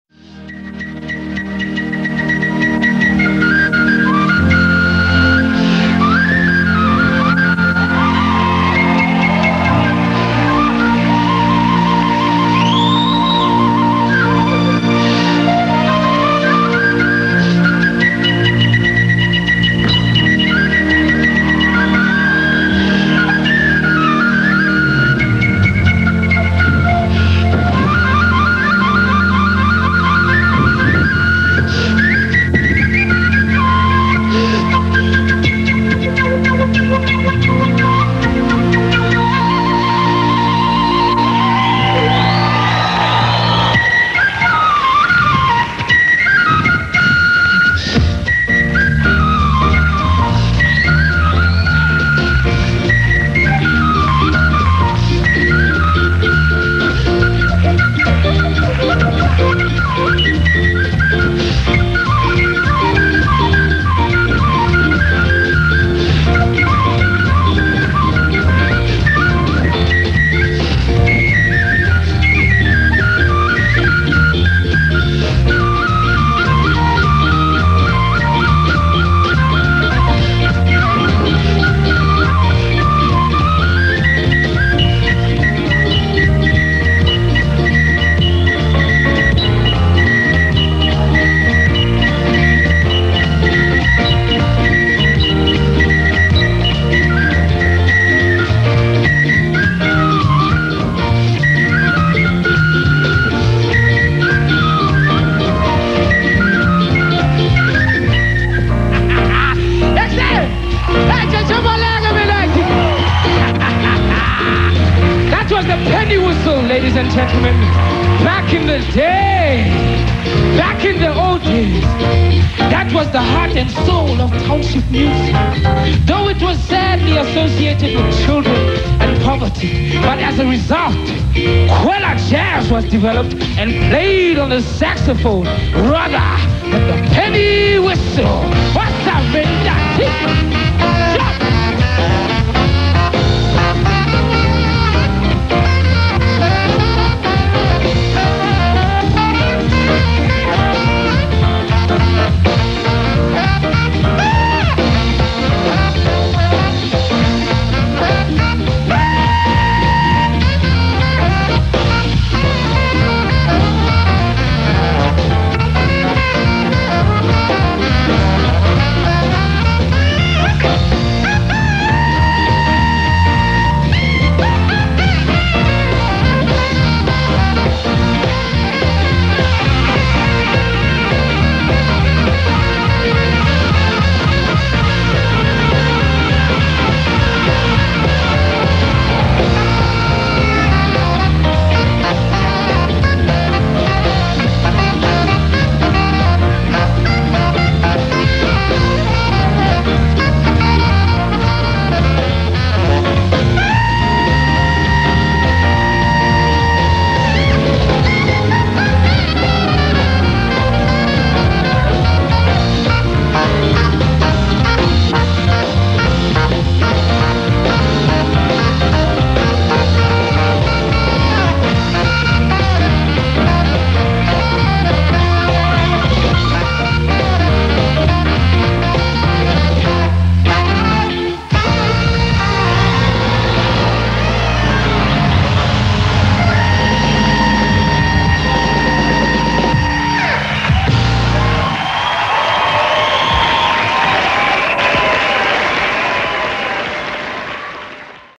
CHRIS LUKE LIVE ON SOUTH AFRICAN TELEVISION (Shaw Bb Tin Whistle from London)
Chris Luke: Live pennywhistle/saxophone performance from South Africa. Yamaha Custom saxophone. Vandoren Jumbo Java mouthpiece A45. Shaw Pennywhistle.